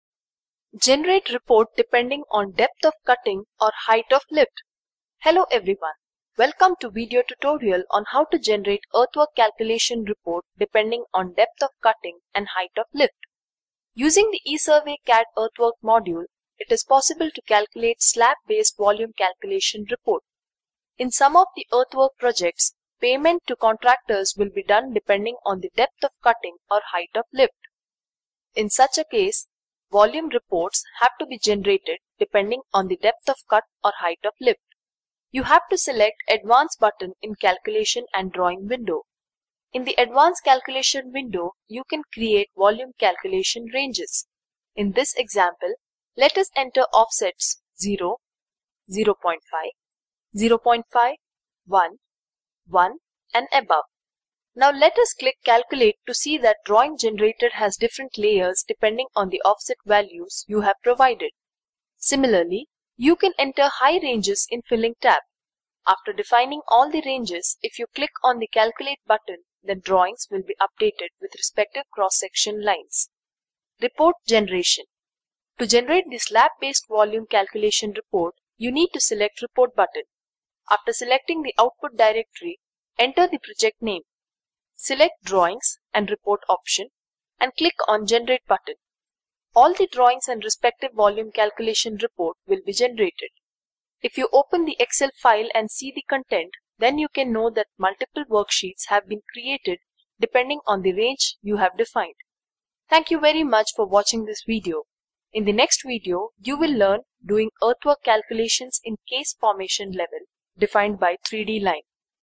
Cut Fill Volume Calculations
ESurvey Earthwork not only gives Volume reports of earthwork for a given set of initial and final levels, using Earthwork Quantity Calculation Software you can generate reports depending on Depth of Cut and Height of Lift. When different kind of Soils is found in Earthwork Projects, this kind of report will be very handy.

Earthwork Software   Depth of Cut and Height of Fill based Volume Report
Hello everyone. Welcome to a video tutorial on how to generate an earthwork calculation report depending on the depth of cutting and Height of lift.

Using the ESurveyCAD earthwork Module it is possible to calculate slab based volume calculation report. In some of the earthwork projects payment to contractors will be done depending on the depth of cutting or height of lift. In such a case, volume reports being generated depending on the depth of cut or height of lift. You have to select the ‘Advanced’ button in Calculation and Drawing window. In the Advance Calculation window, you can create volume calculation ranges. In this example, let us enter offsets 0 – 0.5, 0.5 – 1, 1 & above. Now, let us click Calculate to see that drawing generated has different layers depending on the offset values you have provided.

Similarly, you can enter height ranges in the ‘filling’ tab. After defining all the ranges if you click on the calculate button, then drawings will be updated with respective cross-section lines.

Report Generation
To generate the slab based volume calculation report, you need to select the Report button. After selecting the output directory, enter the project name. Select drawings and Reports option and click on Generate Button. All the drawings and respective volume calculation report will be generated. If you open the excel file and see the content, then you can know that multiple worksheets have been created depending on the range you have defined.
Thank you very much for watching this video. In the next video, you will learn to do earthwork calculations in case the formation level is defined by the 3D line.